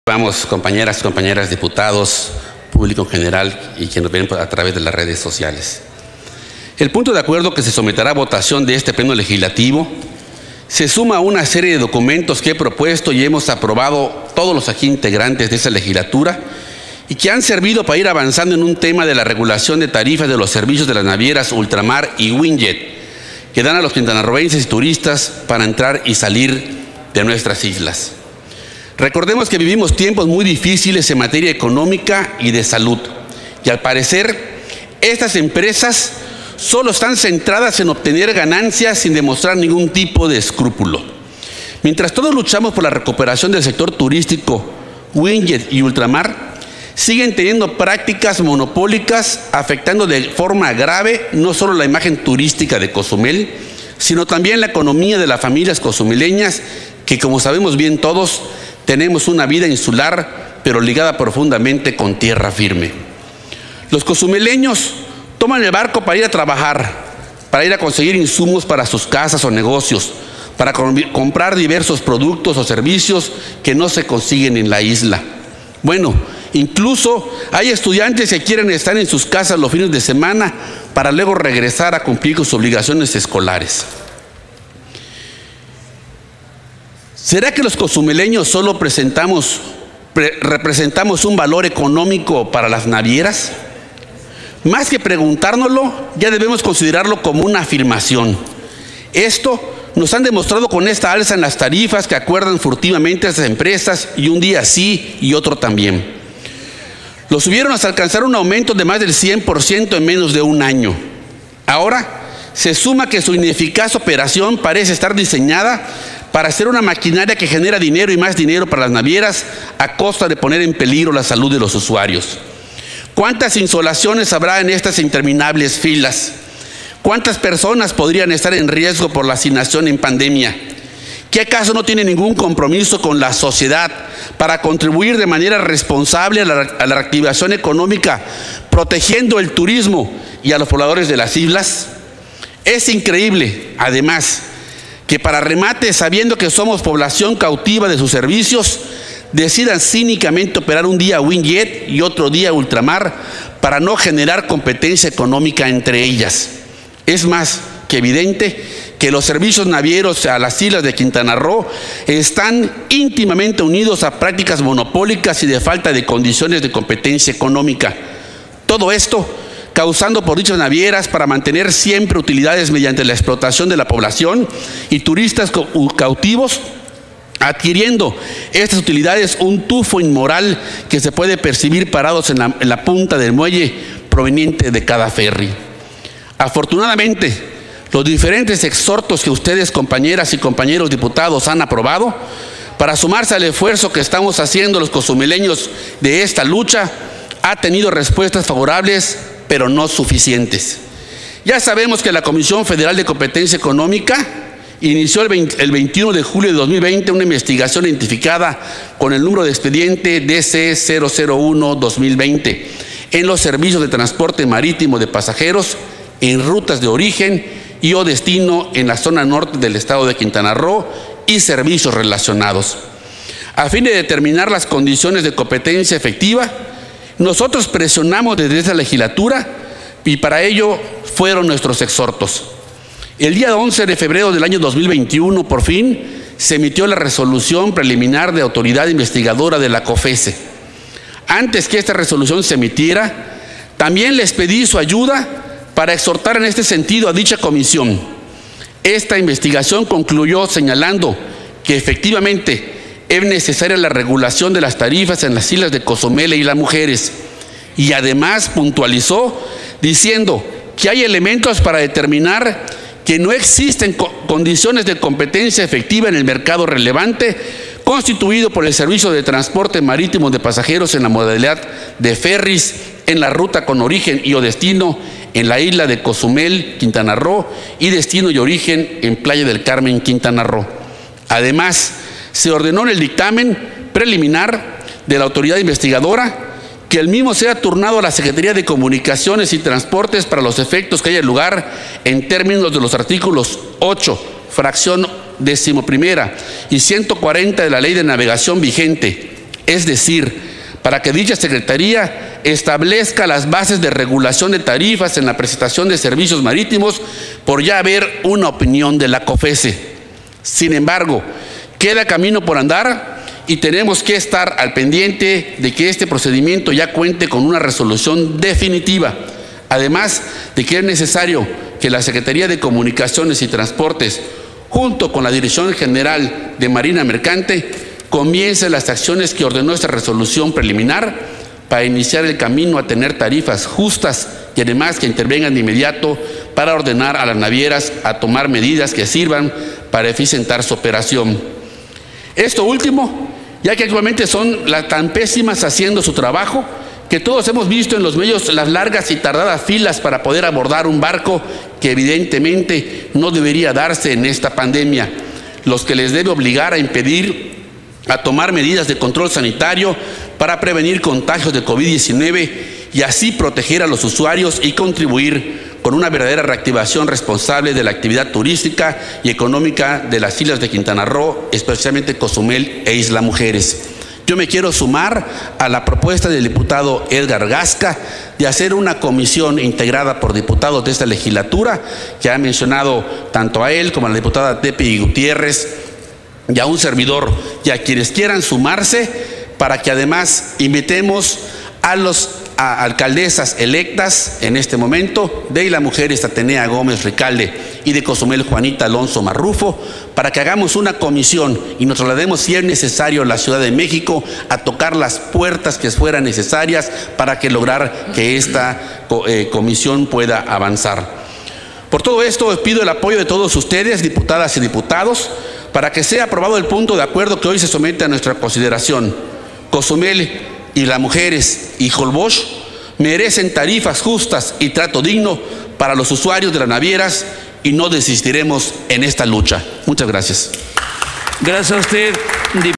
Intervención del Dip. Carlos Rafael Hernández Blanco. PRI (4/Septiembre/2021)
Intervención del Dip. @ del # en relación a la Proposición con Punto de Acuerdo de urgente y obvia resolución, por el que la H. XVI Legislatura del Estado de Quintana Roo, exhorta respetuosamente al Titular de la Secretaría de Comunicaciones y Transportes y al Titular de su dirección general de Marina Mercante, a fin de que comiencen las acciones tendientes a la regulación de las tarifas del servicio de transporte marítimo de pasajeros; así como para que ordenen todas las acciones y medidas necesarias para que en la prestación de dicho servicio en el territorio de Quintana Roo, con especial atención a los traslados entre la Isla de Cozumel y la Ciudad de Playa del Carmen, las navieras ULTRAMAR Y WINJET procedan a establecer medidas de carácter urgente para que durante la actual pandemia de la enfermedad COVID 19, se lleve a cabo ese servicio de manera eficiente, sin aglomeraciones y con las debidas medidas sanitarias para que exista seguridad en el cuidado de la salud de los pasajeros; y además, del mismo modo se exhorta respetuosamente al Pleno de la Comisión Federal de Competencia Económica, a fin de que en ejercicio de sus facultades, continúe con su procedimiento dentro del expediente DC-001-2020 y emita la resolución correspondiente, ratificando el dictamen preliminar, a fin de que la autoridad coordinadora de sector, proceda a establecer definitivamente la regulación de las tarifas y con ello se detenga el abuso de las navieras que operan el transporte marítimo de pasajeros en Quintana Roo; presentada por el Diputado Carlos Rafael Hernández Blanco, Representante Legislativo del Partido Revolucionario Institucional y Presidente de la Comisión de Turismo y Asuntos Internacionales de la XVI Legislatura del Estado; y de la Proposición con Punto de Acuerdo de obvia y urgente resolución, por medio de la cual se exhorta respetuosamente al Pleno de la Comisión Federal de Competencia Económica, por conducto de su Comisionado Presidente, para que el ámbito de sus atribuciones, ordene el dictado de la resolución del Expediente DC-001-2020 en el que la autoridad investigadora dictaminó de manera preliminar que no existen condiciones de competencia efectiva en la prestación del servicio de transporte marítimo de pasajeros en la modalidad de ferris en las rutas Playa del Carmen-Cozumel e Isla Mujeres-Cancún, del Estado de Quintana Roo; presentada por el Diputado Edgar Humberto Gasca Arceo, Presidente de la Comisión de Salud y Asistencia Social de la XVI Legislatura del Estado.